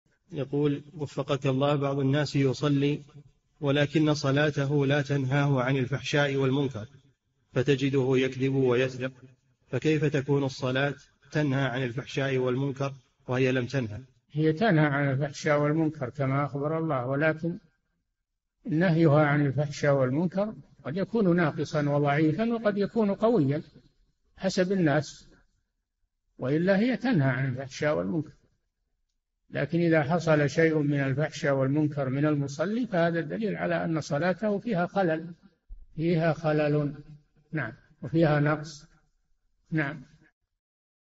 [3396- 4717] كيف الجمع بين كون الصلاة تنهى عن الفحشاء والمنكر وبين وجود المنكر من بعض المصلين؟
"[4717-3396] بعض الناس يصلي ولكن صلاته لا تنهاه عن الفحشاء والمنكر فتجده يكذب ويسرق فكيف تكون الصلاة تنهى عن الفحشاء والمنكر وهي لم تنهه؟
الجَوَابُ: هي تنهى عن الفحشاء والمنكر كما أخبر الله ولكن نهيها عن الفحشاء والمنكر قد يكون ناقصًا وضعيفًا وقد يكون قويًا حسب الناس وإلا هي تنهى عن الفحشاء والمنكر لكن إذا حصل شيء من الفحشاء والمنكر من المصلي فهذا دليل على أنّ صلاته فيها خلل وفيها نقص.

للاشتراك في قناة الشيخ صالح الفوزان
فتاوى المجالس في تفسير المفصل

للاستفسار والإقتراحات والدعم
رئيس الفريق
لمشروع كبار العلماء